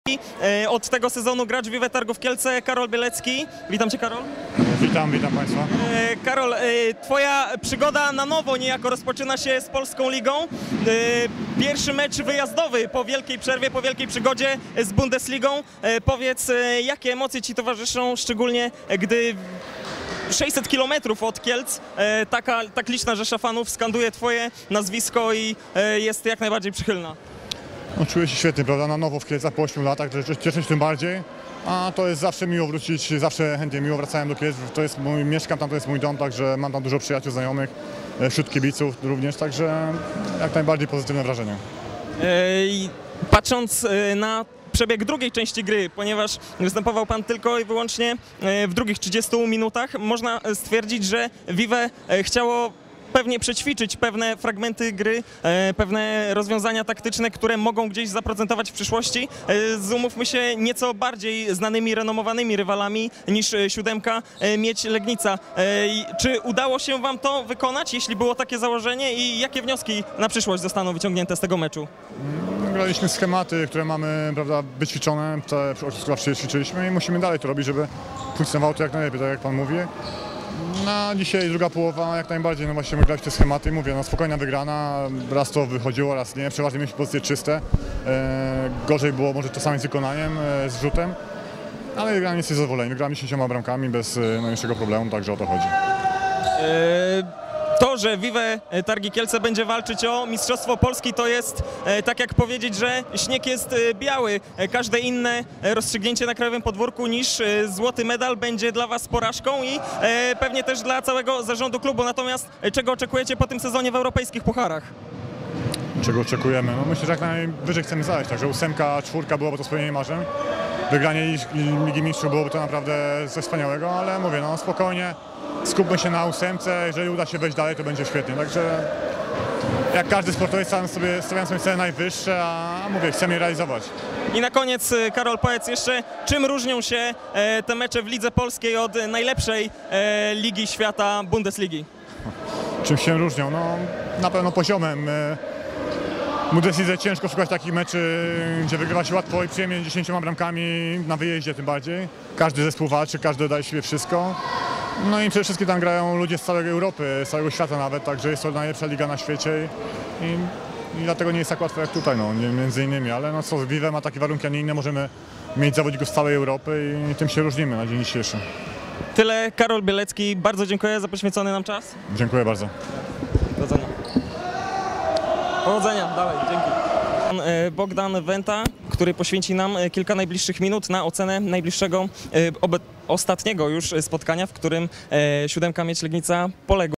Karol Bielecki wywiad w Legnicy
Karol Bielecki po wygranej z Siódemką Legnica udzielił wywiadu portalowi legniczanin.pl